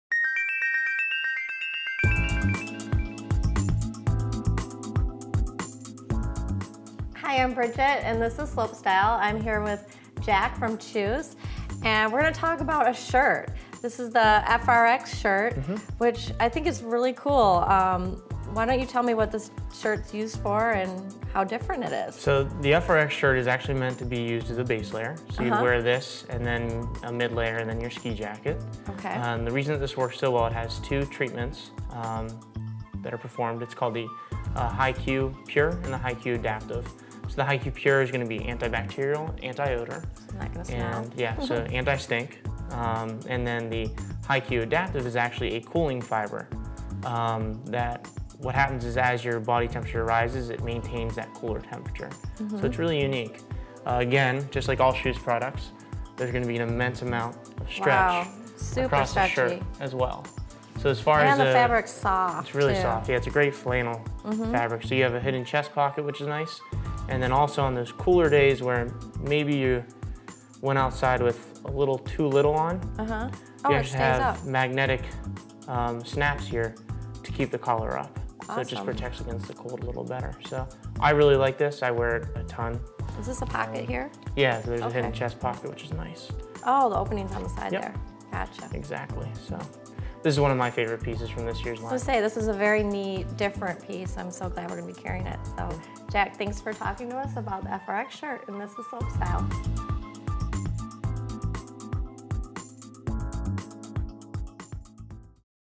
2016 Kjus FRX Mens Shirt Overview by SkisDotCom
The KJUS FRX Mens Flannel Shirt expresses the revival of back country skiing. Combining casual country-plaid with cutting-edge technology is the result of The FRX mens Shirt. With the look of (layed-back) styling and innovative winter protection, The FRX Plaid Flannel Shirt provides maximum wearing comfort. The adaptive fabric finish delivers the perfect temperature regulation. The cozy, relaxed design and feel of The FRX Shirt is the ideal wearable for the natural outdoorsmen.